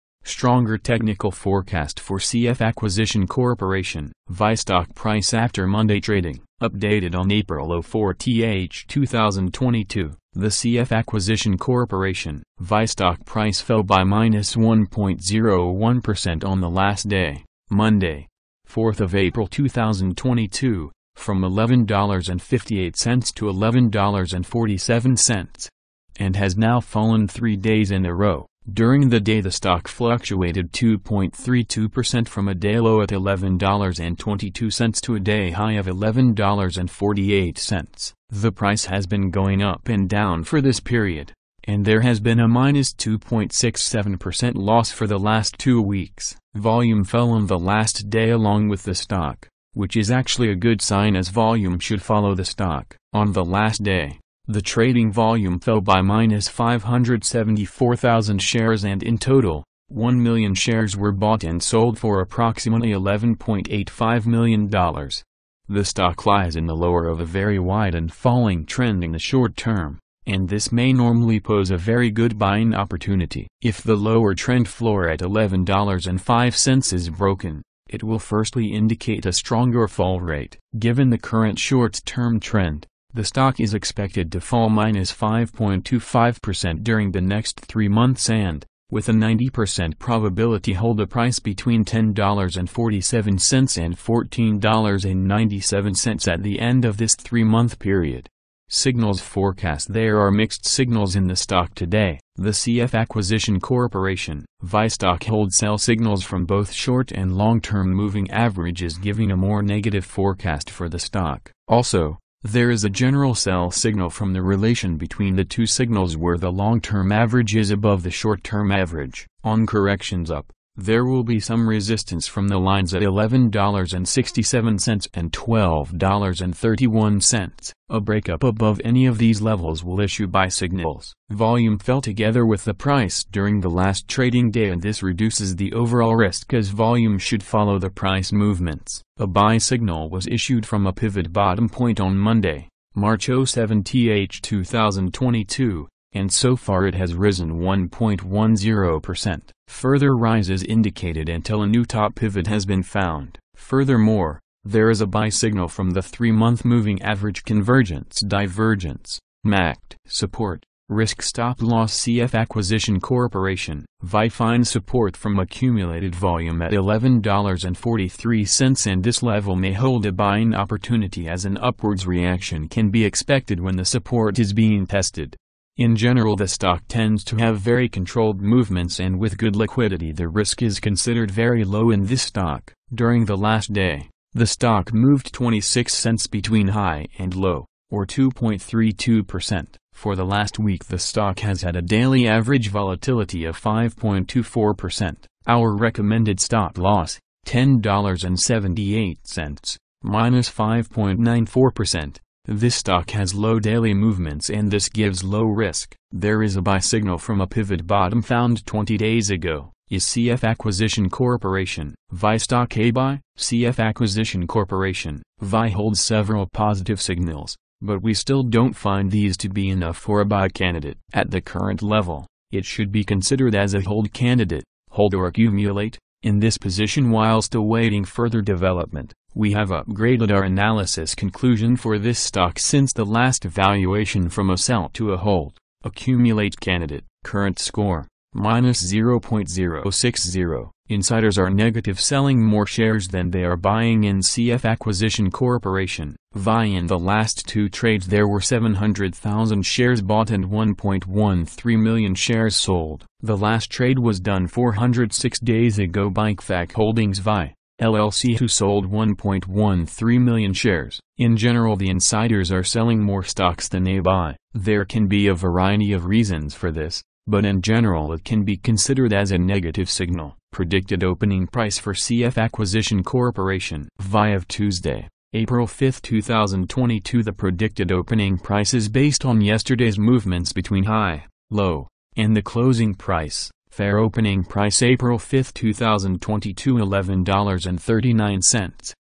CF Acquisition Corp VI 💰 Cfvi stock . 5 april 2022 🚀 Cfvi stock news 🤑!!
Updated on Apr 05, 2022

Stronger technical forecast for CF Acquisition Corp. VI stock price after Monday trading.
(Updated on Apr 04, 2022)

The CF Acquisition Corp. VI stock price fell by -1.01% on the last day (Monday, 4th Apr 2022) from $11.58 to $11.47. and has now fallen 3 days in a row. During the day the stock fluctuated 2.32% from a day low at $11.22 to a day high of $11.48. The price has been going up and down for this period, and there has been a -2.67% loss for the last 2 weeks. Volume fell on the last day along with the stock, which is actually a good sign as volume should follow the stock. On the last day, the trading volume fell by -574 thousand shares and in total, 1 million shares were bought and sold for approximately $11.85 million.

The stock lies in the lower of a very wide and falling trend in the short term, and this may normally pose a very good buying opportunity. If the lower trend floor at $11.05 is broken, it will firstly indicate a stronger fall rate. Given the current short-term trend, the stock is expected to fall -5.25% during the next 3 months and, with a 90% probability hold a price between $10.47 and $14.97 at the end of this 3-month period.

Signals & Forecast
There are mixed signals in the stock today. The CF Acquisition Corp. VI stock holds sell signals from both short and long-term moving averages giving a more negative forecast for the stock. Also, there is a general sell signal from the relation between the two signals where the long-term average is above the short-term average. On corrections up, there will be some resistance from the lines at $11.67 and $12.31. A break-up above any of these levels will issue buy signals. Volume fell together with the price during the last trading day and this reduces the overall risk as volume should follow the price movements. A buy signal was issued from a pivot bottom point on Monday, March 07, 2022, and so far it has risen 1.10%. Further rise is indicated until a new top pivot has been found. Furthermore, there is a buy signal from the 3 month Moving Average Convergence Divergence (MACD).

Support, Risk & Stop-loss
CF Acquisition Corp. VI finds support from accumulated volume at $11.43 and this level may hold a buying opportunity as an upwards reaction can be expected when the support is being tested.

In general the stock tends to have very controlled movements and with good liquidity the risk is considered very low in this stock. During the last day, the stock moved $0.26 between high and low, or 2.32%. For the last week the stock has had a daily average volatility of 5.24%.

Our recommended stop-loss: $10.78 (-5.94%) (This stock has low daily movements and this gives low risk. There is a buy signal from a pivot bottom found 20 days ago.)

Is CF Acquisition Corp. VI stock A Buy?
CF Acquisition Corp. VI holds several positive signals, but we still don't find these to be enough for a buy candidate. At the current level, it should be considered as a hold candidate (hold or accumulate) in this position whilst awaiting further development. We have upgraded our analysis conclusion for this stock since the last evaluation from a Sell to a Hold/Accumulate candidate.

Current score: -0.060

Insiders are negative selling more shares than they are buying in CF Acquisition Corp. VI
In the last 2 trades there were 700 thousand shares bought and 1.13 million shares sold. The last trade was done 406 days ago by Cfac Holdings Vi, Llc who sold 1.13 million shares. In general the insiders are selling more stocks than they buy. There can be a variety of reasons for this, but in general it can be considered as a negative signal.

Predicted Opening Price for CF Acquisition Corp. VI of Tuesday, April 5, 2022
The predicted opening price is based on yesterday's movements between high, low, and the closing price.

Fair opening price April 5, 2022
$11.39